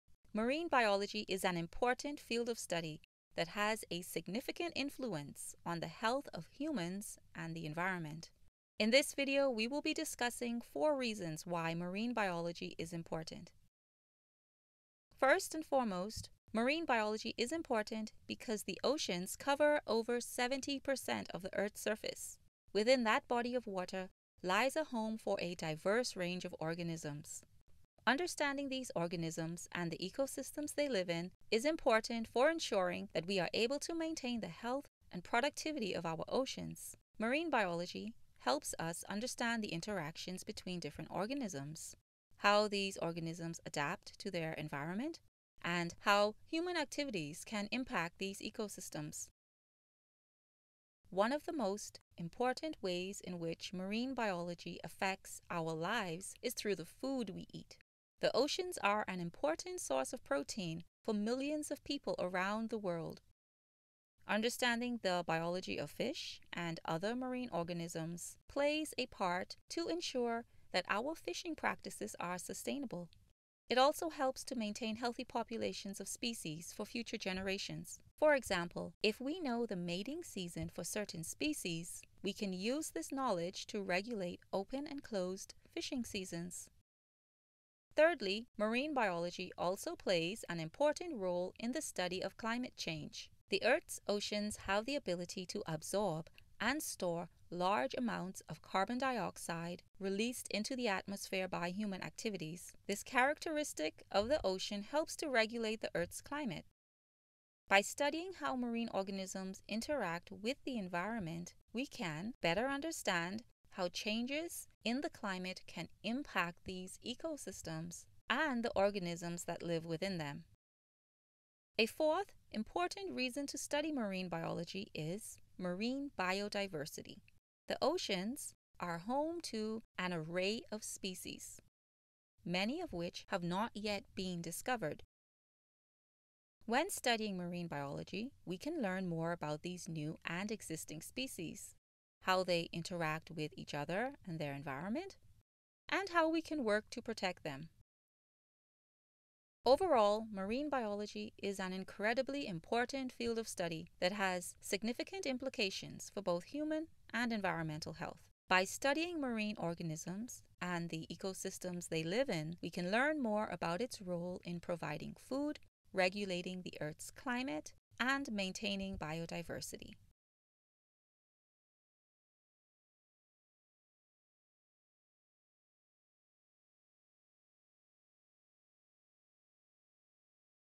Why is Marine Biology Important? | Explore Marine Biology
Marine biology is an important field of study that has a significant influence on the health of humans and the environment. In this video, we will be discussing four (4) reasons why marine biology is important.

Our new free course is available!!